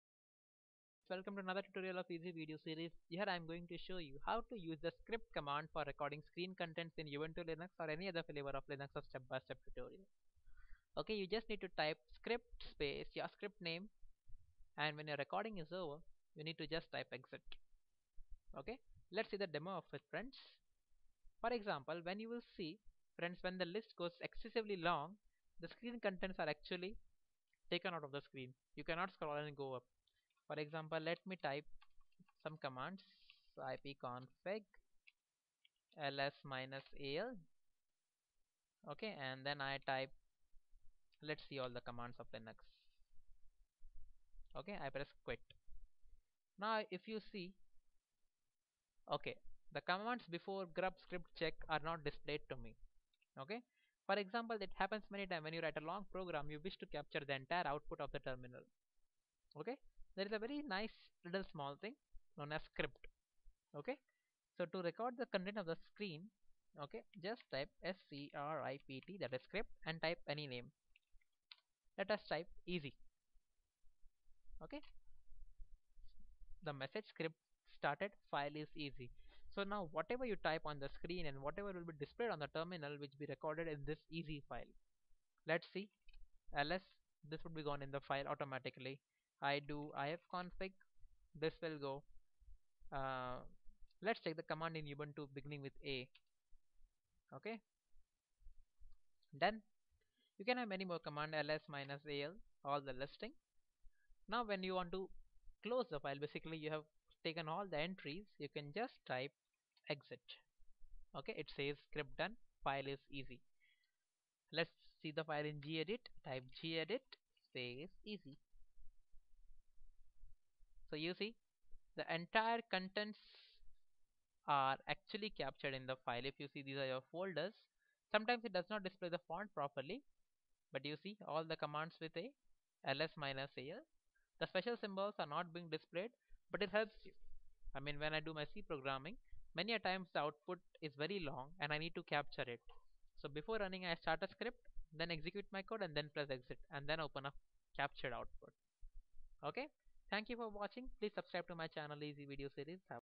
How To use the script command for recording screen contents in Ubuntu Linux Tutorial
How To use the script command for recording screen contents in Ubuntu Linux Via Command Line Or Terminal Step By Step Tutorial